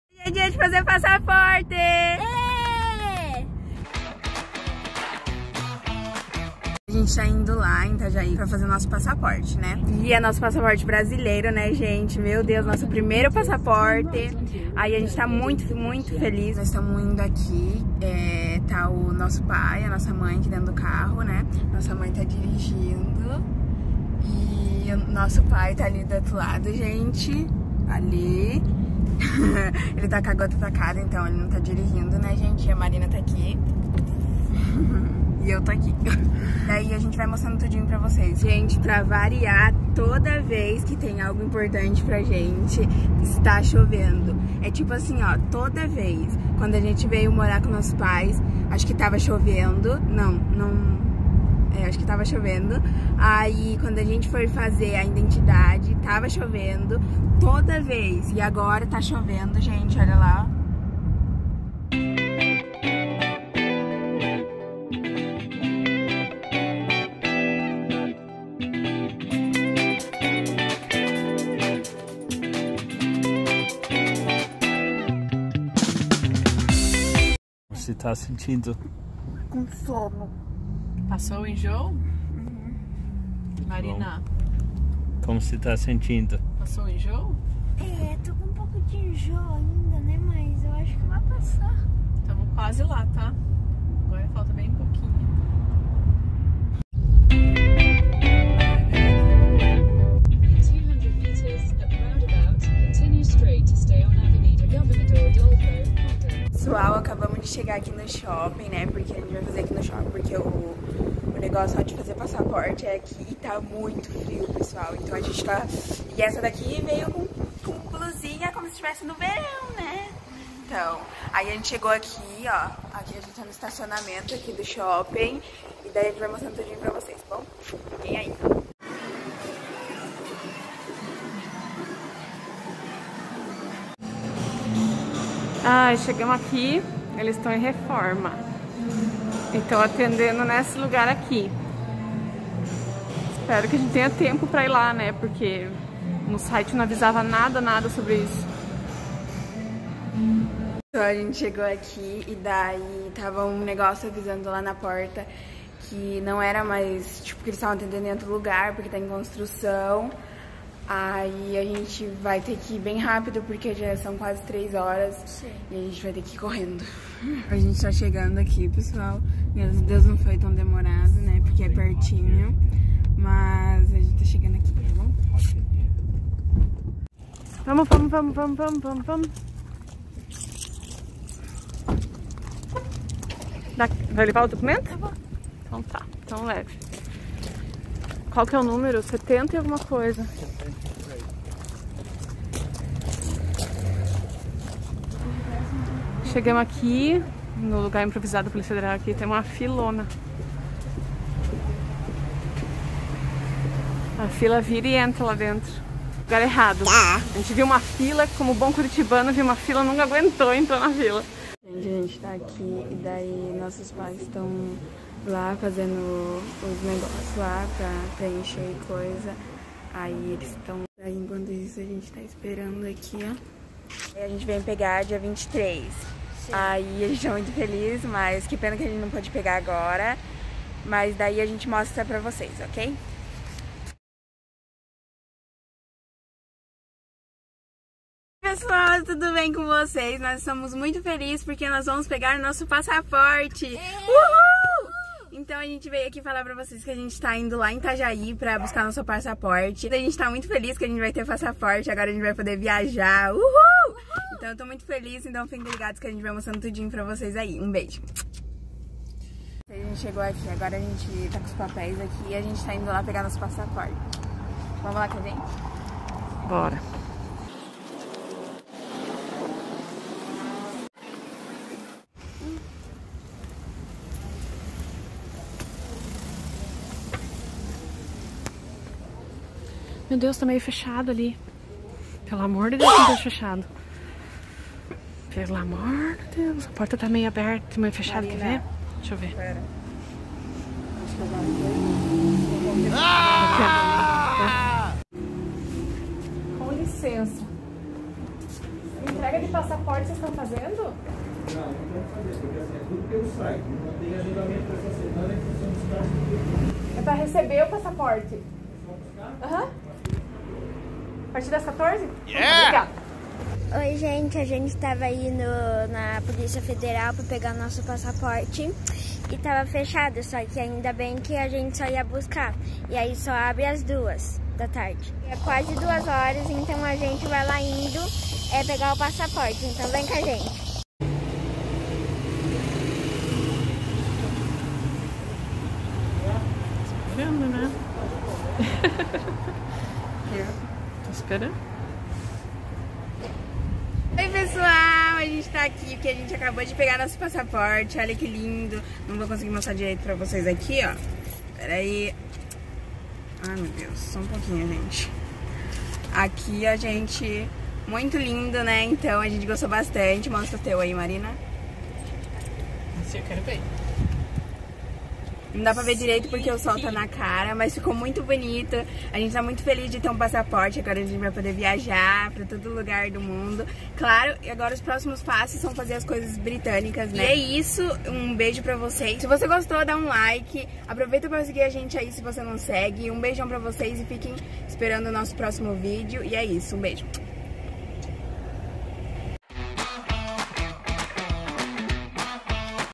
Passaporte Brasileiro - Check
Estamos muito felizes por ter conseguido fazer todos os documentos Brasileiros.
Para fazer o Passaporte Brasileiro fomos para Itajaí, tivemos alguns contratempos por lá, mas deu tudo certo no final!